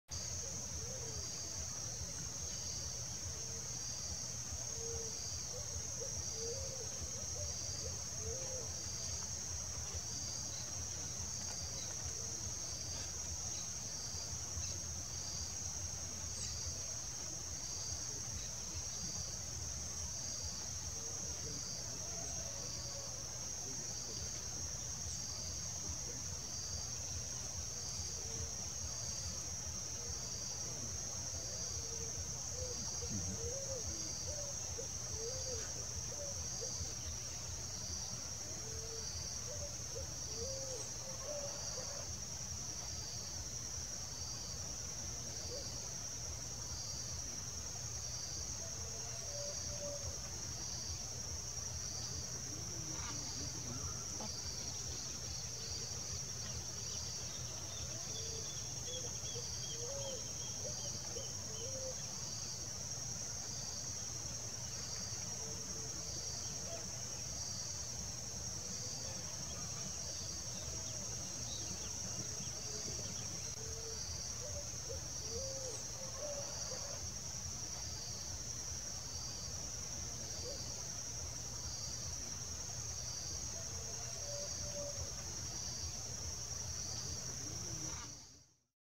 HARLEQUIN, HYBRID MACAW, RARE, BEAUTIFUL AND COLORFUL BIRD LIVING IN COMPLETE FREEDOM IN THE WILD.
Hybrid animal resulting from the cross between two different species: (Red-and-green Macaw (Ara chloropterus)  x Blue-and-yellow Macaw (Ara ararauna)). Such interbreeding is happening naturally among the animals, and is already worrying local environmentalists. On the other hand, a new species of bird may be emerging in the wild.